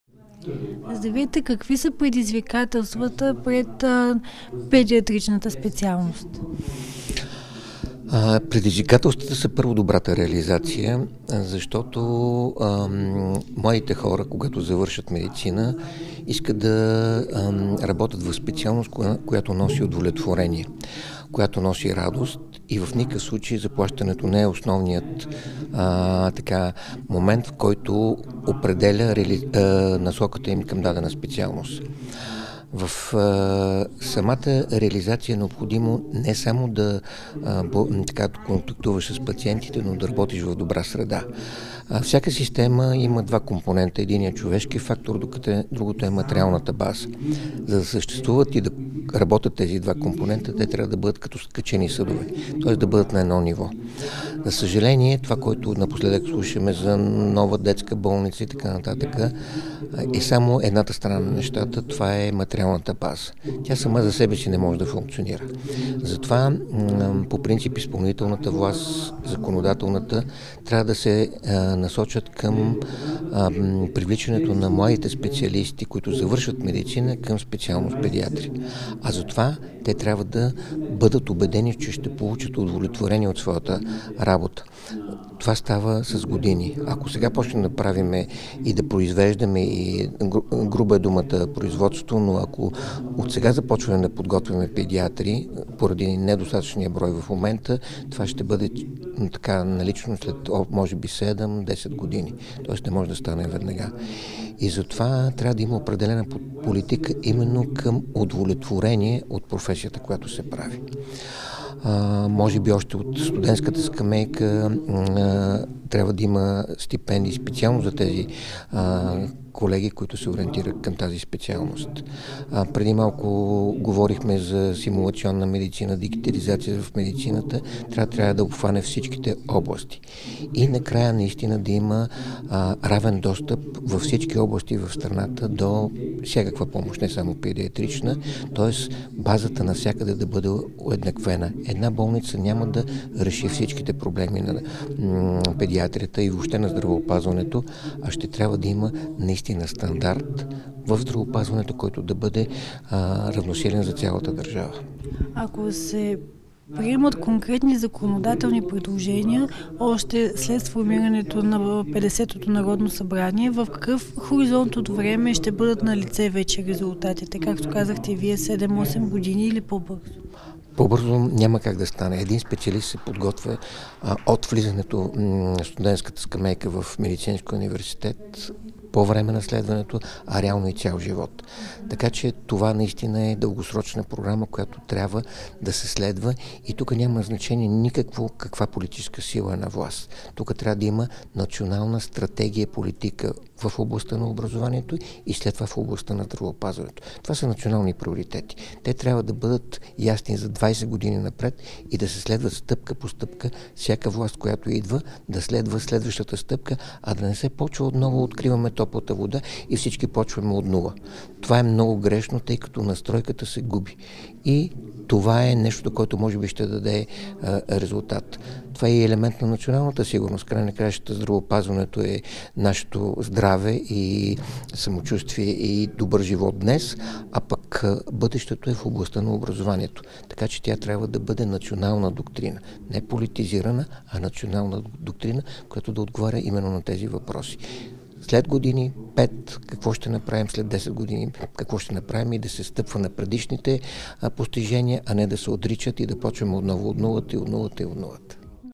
Проф. д-р Любомир Спасов: Необходима е държавна политика за педиатрите
Човешкият фактор и материалната база в здравеопазването са като скачени съдове. Ако искаме системата да работи добре, те трябва да са на едно ниво. Това заяви проф. Любомир Спасов - декан на Медицински университет в СУ, и бивш директор на болница "Лозенец". I БГНЕС